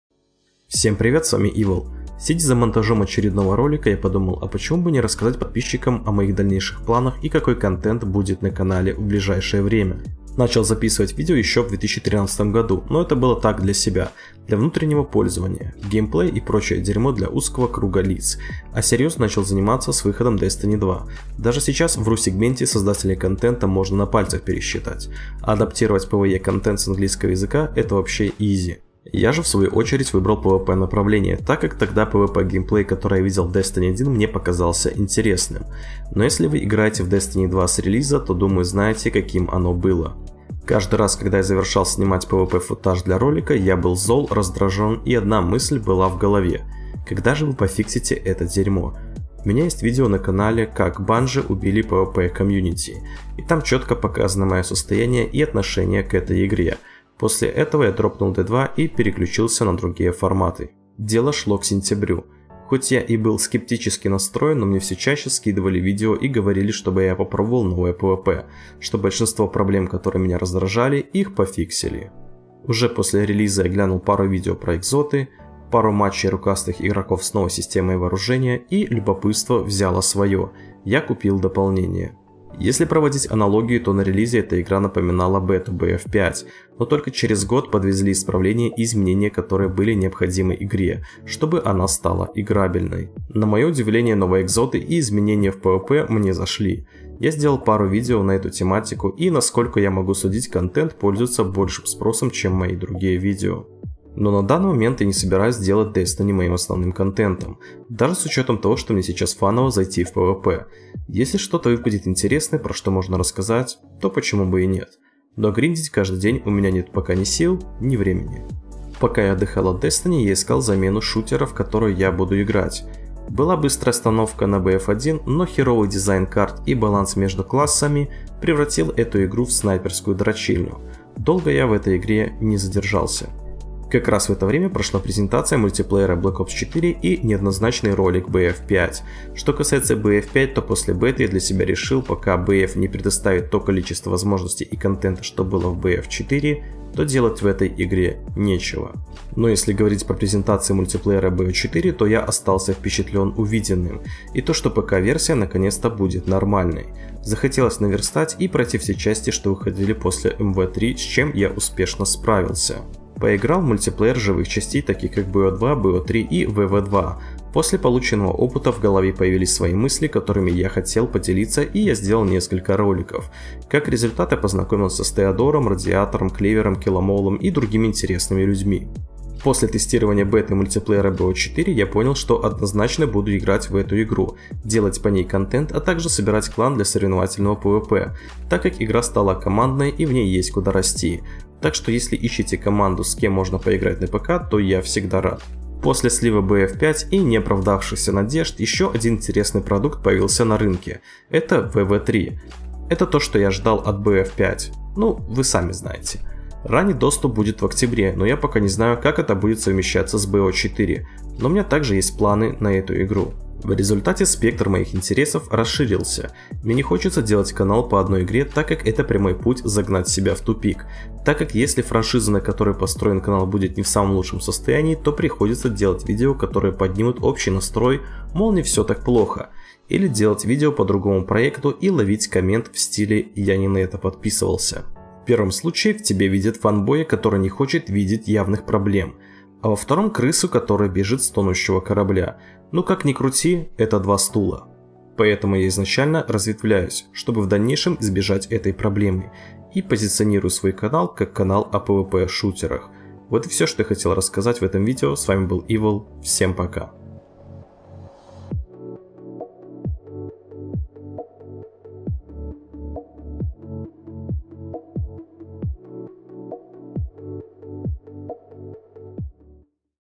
Подкаст о канале и контенте
Подкаст о текущей ситуации и моих будущих планах